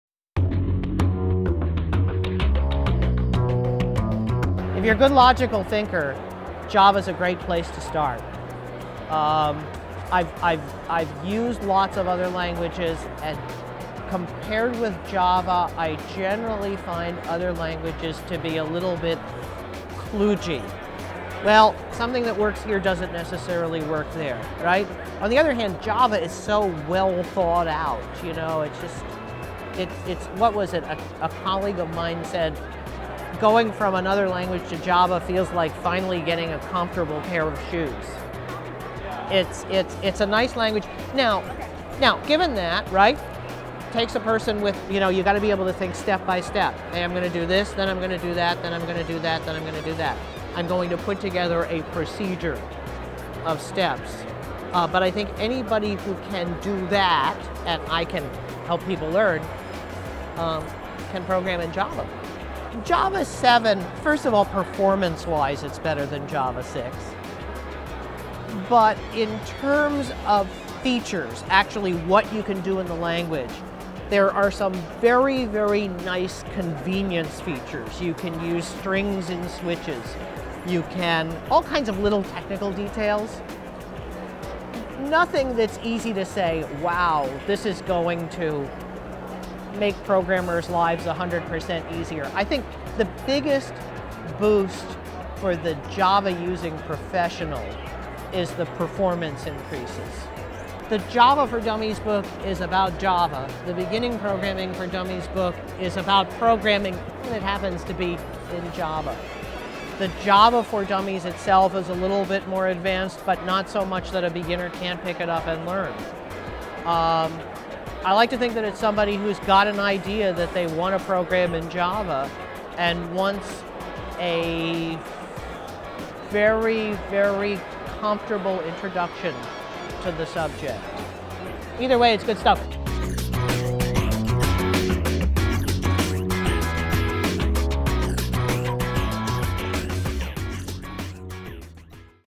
Java For Dummies Barry Burd
Bestselling Java For Dummies author Barry Burd talks about the release of Java 7, offering tips for programming with java.